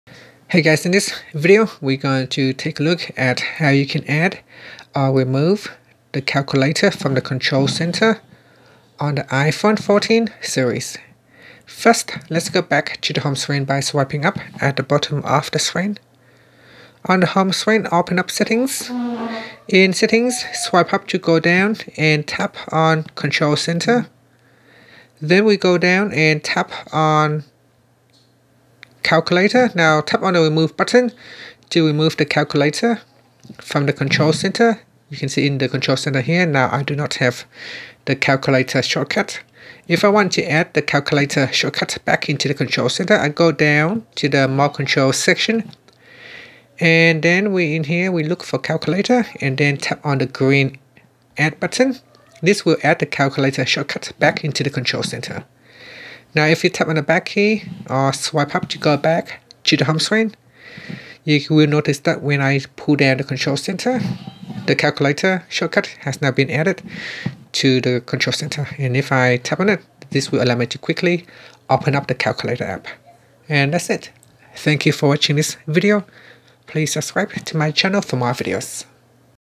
iPhone 14's/14 Pro Max: How to Add/Remove The Calculator From The Control Center
Learn how you can add or remove the calculator from the control center on the iPhone 14/14 Pro/14 Pro Max/Plus.